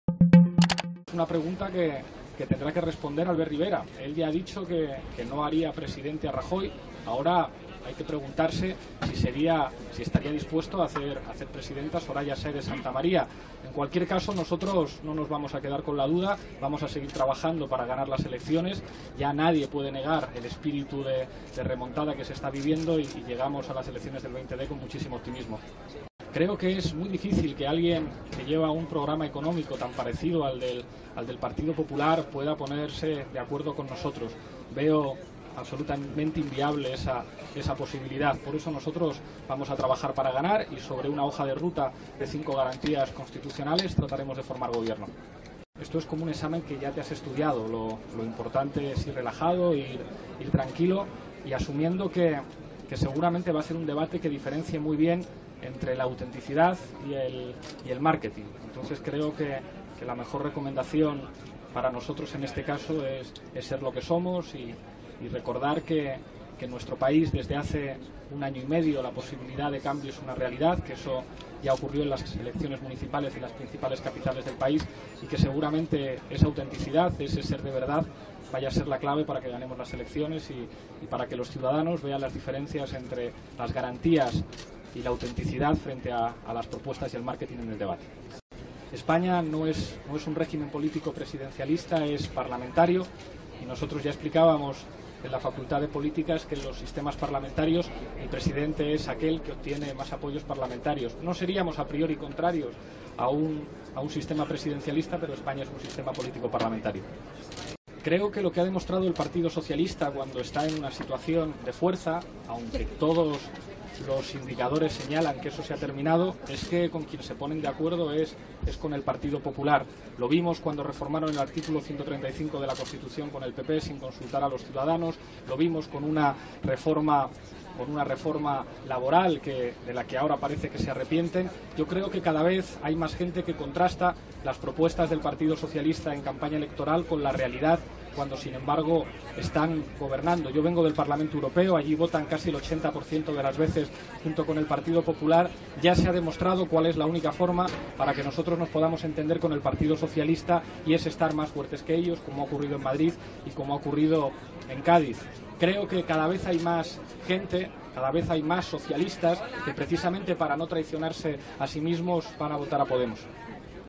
Iglesias emplaza a Rivera a aclarar si apoyaría a Santamaría de presidenta
Madrid, 6 dic (EFE).- El candidato de Podemos a la presidencia del Gobierno, Pablo Iglesias, ha emplazado hoy al presidente de Ciudadanos, Albert Rivera, a aclarar si estaría dispuesto a apoyar a la vicepresidenta, Soraya Sáenz de Santamaría, jefa del Ejecutivo.

Iglesias ha respondido así a los periodistas a su llegada al Congreso para el acto institucional del día de la Constitución, al ser preguntado por la "operación Menina", que él mismo atribuyó ayer al PP para hacer presidenta a Sáenz de Santamaría con el apoyo de Rivera.

"Esa pregunta la tendrá que responder Rivera; ya ha dicho que no haría presidente a Rajoy, ahora tiene que aclarar si estaría dispuesto a hacer presidenta a Soraya Sáenz de Santamaría", ha indicado.

En cualquier caso, ha asegurado que Podemos no se va a quedar con la duda y va a seguir trabajando para ganar las elecciones del 20 de diciembre, ante las que ha mostrado "muchísimo optimismo".

"Nadie puede negar que estamos viviendo un espíritu de remontada", ha dicho.

El secretario general de Podemos ha explicado que "por desgracia" no pudo ver el programa televisivo en La Sexta en el que participó ayer el presidente del Gobierno, pero ha avanzado que hoy buscará un rato para ver a Rajoy.

Iglesias, que piensa que "es una buena noticia" que Rajoy "haya dado la cara ante la gente", aunque no se encontrara "demasiado cómodo", también ha recordado que Podemos es partidario de que la presencia de los presidentes y los candidatos sean una obligación.

Ante la pregunta de si debe gobernar el candidato más votado en las próximas elecciones, Iglesias ha recordado que España no es un régimen político presidencialista, sino parlamentario, y el presidente es el que recibe el mayor apoyo parlamentario.

Sobre un posible pacto con los socialistas, ha insistido en que se ha demostrado que la única forma de entenderse con el PSOE es cuando Podemos está más fuerte que ellos, como ha ocurrido en Madrid o en Cádiz.

Y, por último, de cara al debate electoral que mantendrá mañana con Sánchez, Rivera y la vicepresidenta Soraya Sáenz de Santamaría, ha dicho que está relajado y tranquilo, porque es como ir a un examen para el que se ha estudiado.

Confía en que sea un debate que deje clara ante los ciudadanos la diferencia entre la "autenticidad y el márketing".

En su opinión, la mejor recomendación que puede recibir para este debate es "ser lo que somos" y recordar al país que lo que comenzó hace año y medio puede convertirse en realidad como ya ocurrió en las elecciones municipales de mayo.

Esa, a su juicio, será "de verdad la clave" para que Podemos gane las elecciones: que los ciudadanos vean las diferencias. EFE

DECLARACIONES DEL CANDIDATO DE PODEMOS A LA PRESIDENCIA DEL GOBIERNO, PABLO IGLESIAS, ANTES DE PARTICIPAR EN EL CONGRESO EN EL ACTO INSTITUCIONAL CON MOTIVO DEL DÍA DE LA CONSTITUCIÓN.